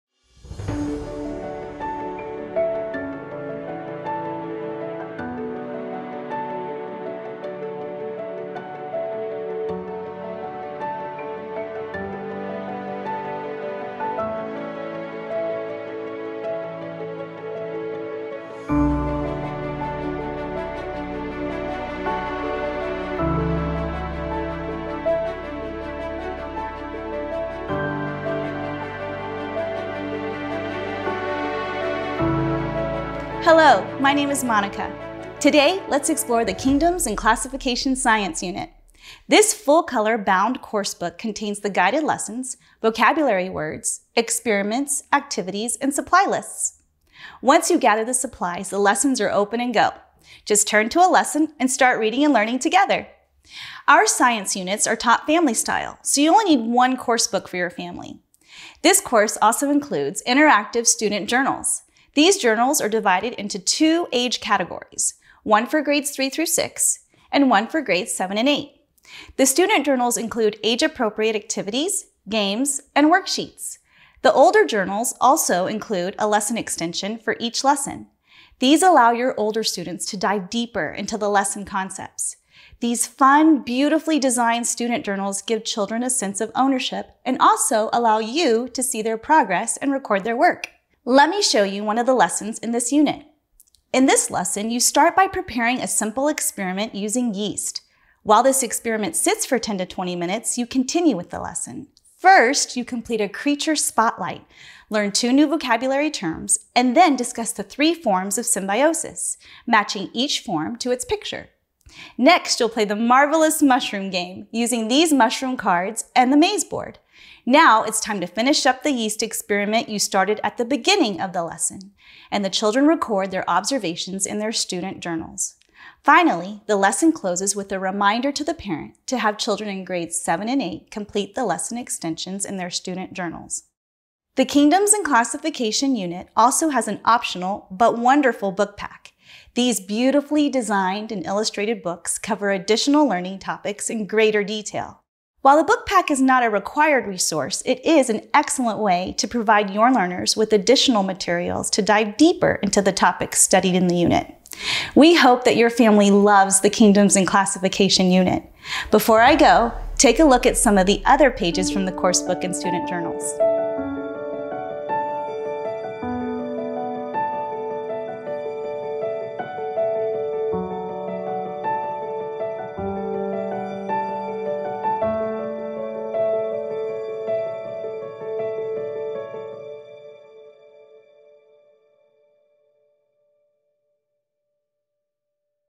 Kingdoms and Classification Homeschool Science | Flip Through | The Good and the Beautiful
Peek into our updated Kingdoms and Classification unit study! Explore animals, plants, fungi, bacteria, and more in 11 no-prep lessons. Brand-new student journals and exciting videos accompany the family-style homeschool science unit. Be sure to check out the read-aloud book pack for even more learning!

(0:00) Introduction
(0:39) The Course Book
(0:58) Student Journals
(1:30) Example Lesson
(2:14) Optional Book Pack
(2:38) Sneak Peek at Additional Pages